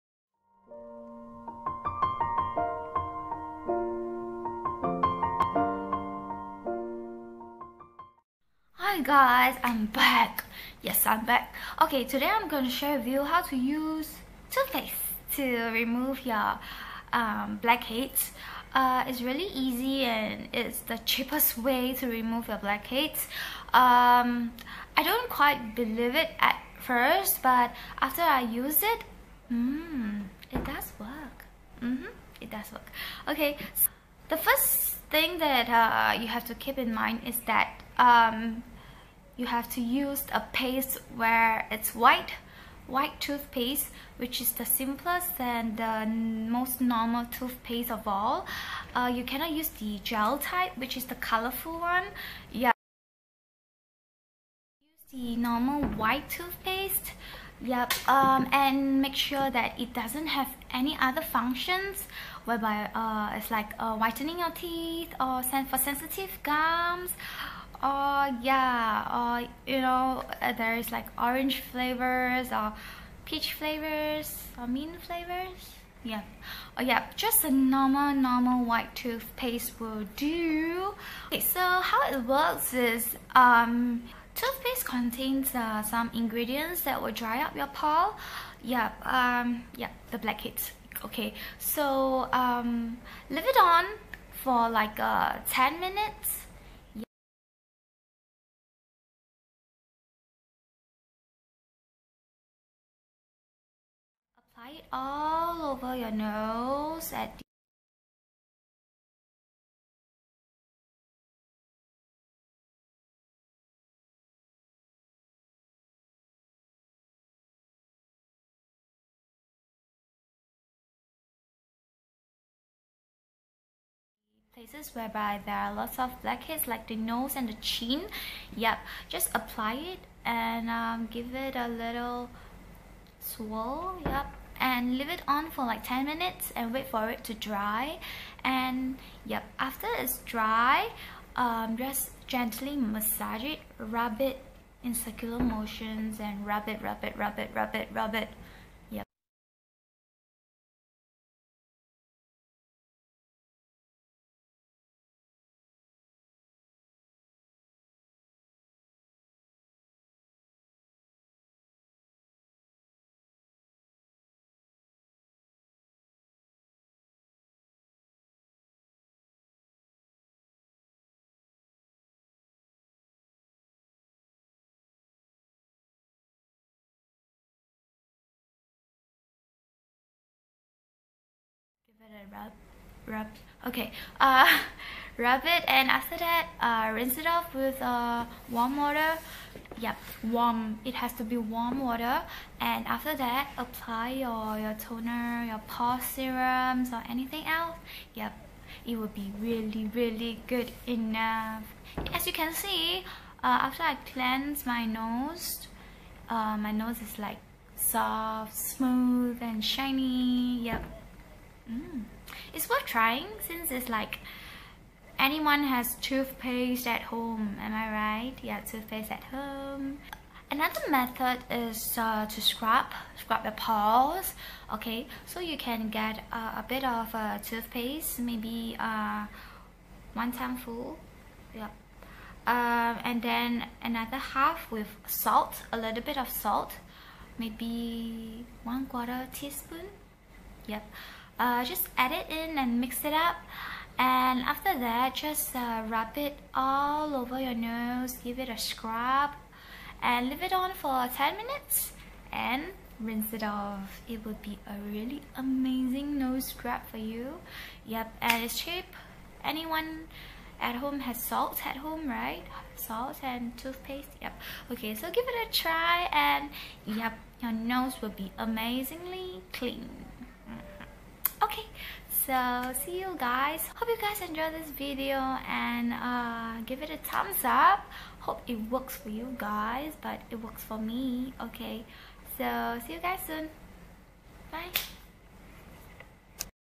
Amazing Cheap Blackheads Removal Tip
Hello Dearies,
Share with you guys a cheap and effective blackheads removal tip.
Watch this video to find out more ya.

Love ya=)
Cheers!!